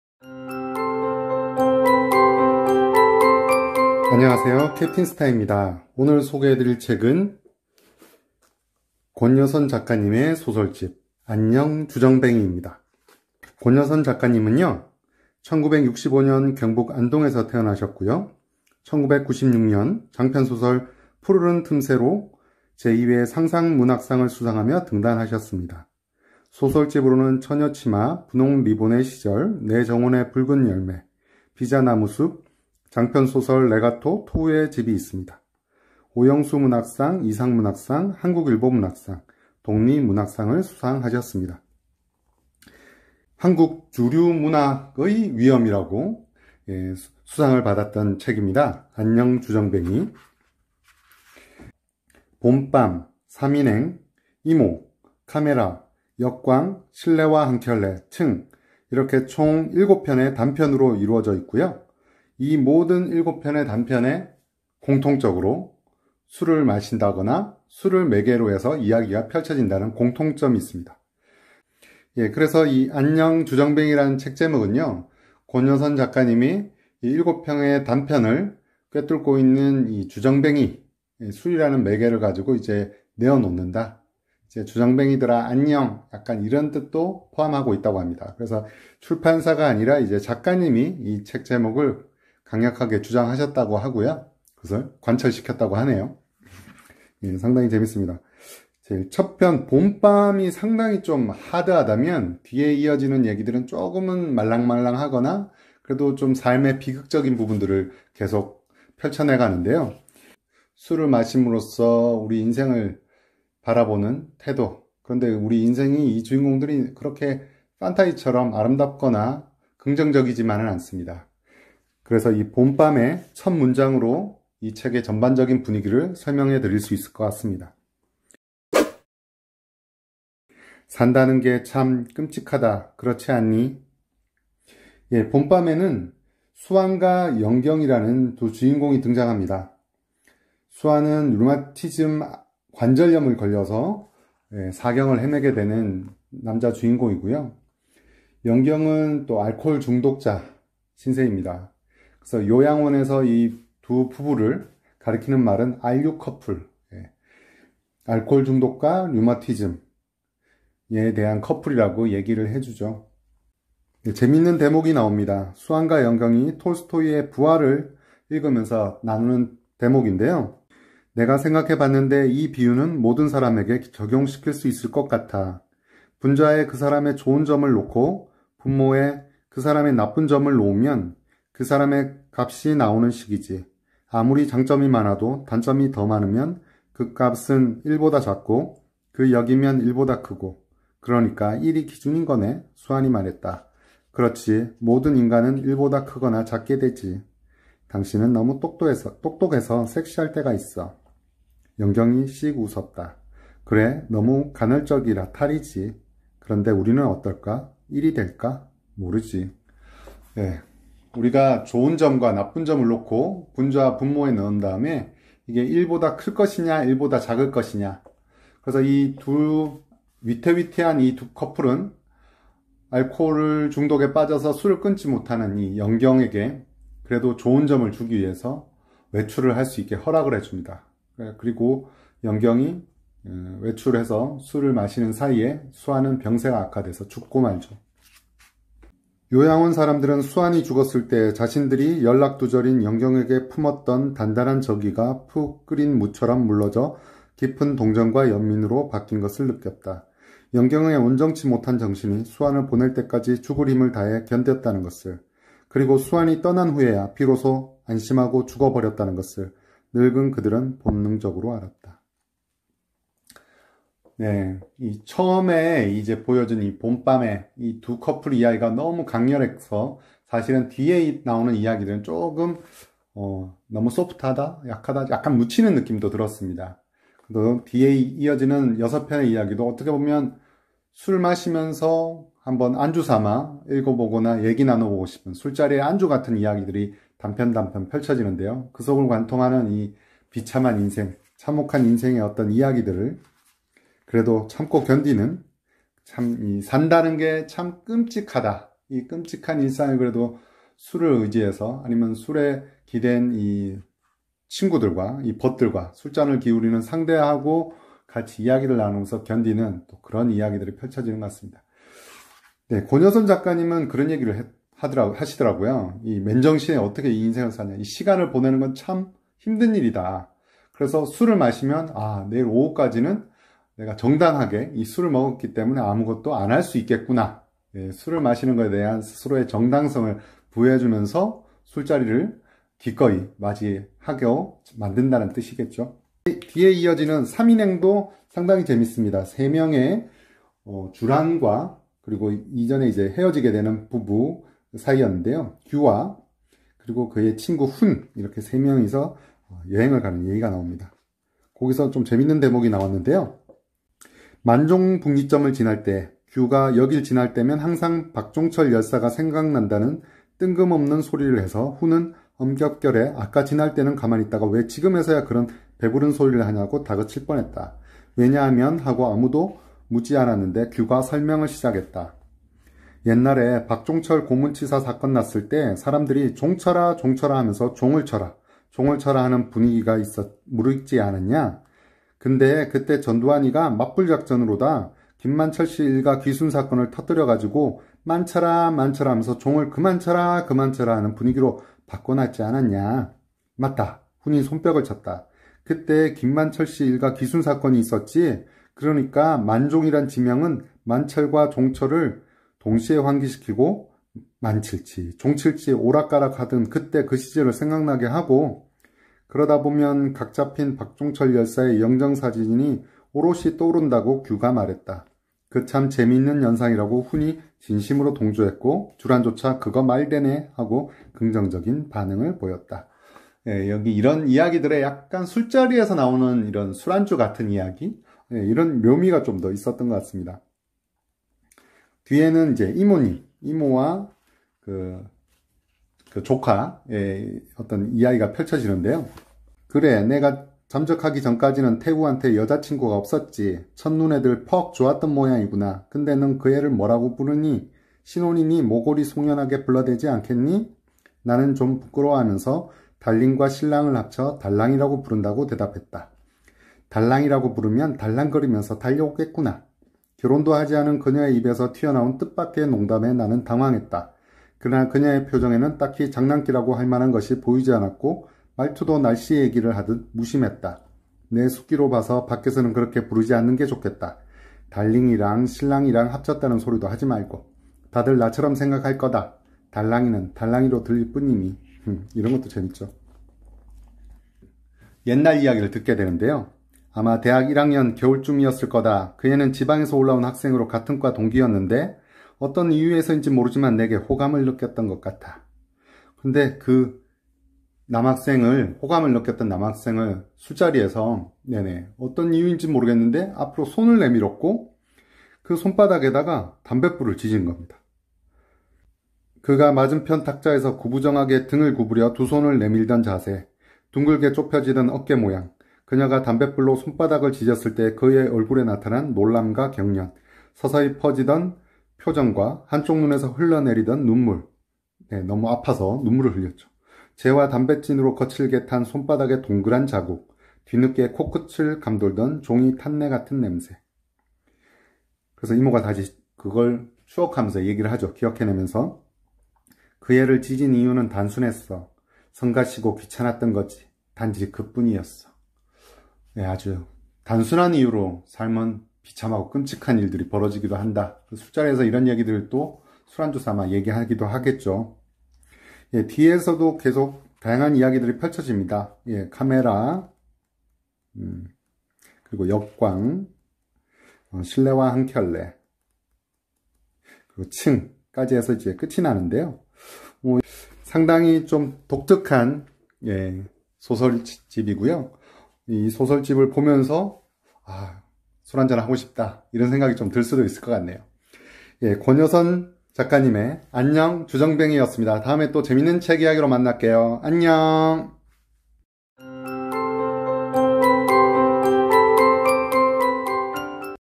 [캡틴스타] (안녕 주정뱅이) 현대 한국 주류 문학의 위엄! 혼술하면서 읽어볼만한 소설집!
안녕하세요? 캡틴스타입니다.

오늘 소개해드릴 책은 [안녕 주정뱅이]입니다.

한국 주류(술주)문학의 위엄!

혼술 하면서 안주 삼아 읽기 좋은 소설집입니다.

코로나19로 다시 집콕하는 시기에, 혼술하면서 읽어볼만한 책으로 추천드립니다.

다시금 코로나 이전처럼 여럿이서 술잔을 기울일 수 있는

시절이 오길 기원하면서~ 안녕 코로나19~

제 지난 영상 목록의 링크는다음과 같습니다.

감사합니다.

소개해드리는 책 구매는 아래에서 하실 수 있습니다.